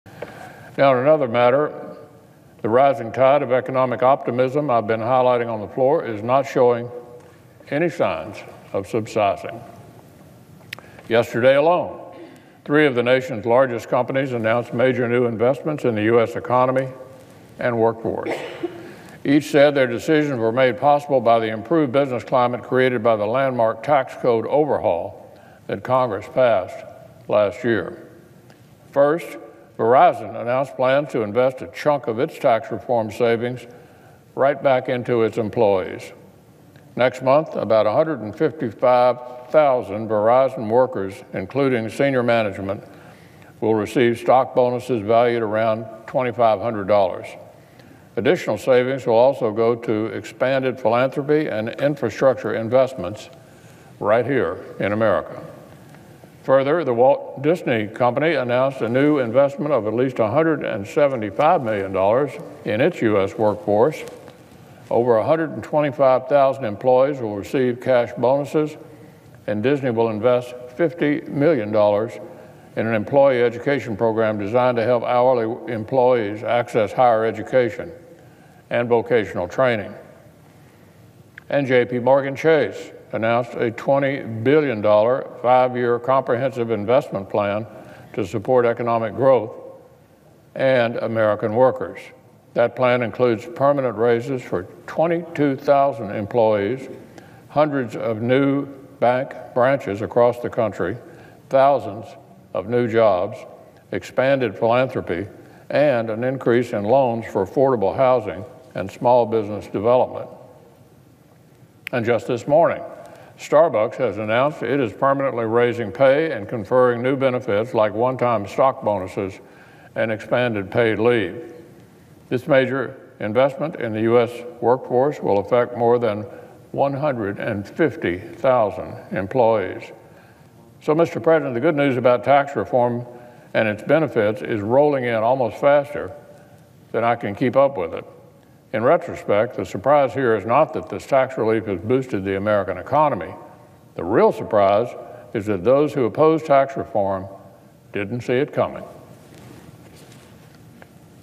Tax Reform Continues to Bring Good News For American Workers, Economy
WASHINGTON, D.C. – U.S. Senate Majority Leader Mitch McConnell (R-KY) delivered the following remarks today on the Senate floor regarding the benefits Americans continue to see across the country thanks to tax relief.